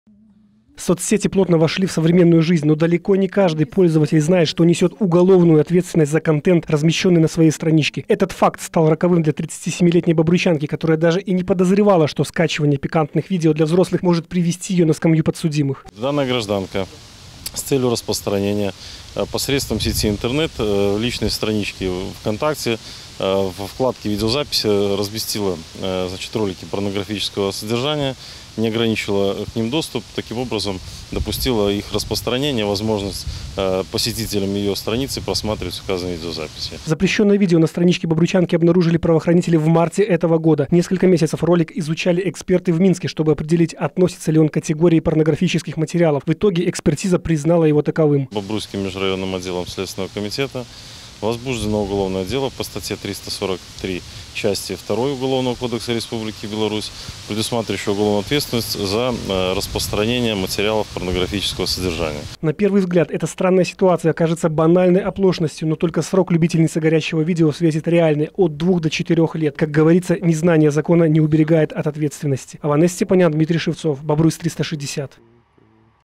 СТАТЬЯ ЗА «КЛУБНИЧКУ»
Убийства, грабежи, кражи, мошенничества – эти преступления регулярно мелькают в оперативных сводках. Люди зачастую совершают их сознательно, как правило, ради наживы, но уголовное дело, заведенное накануне в отношении жительницы Бобруйска можно назвать преступлением по глупости.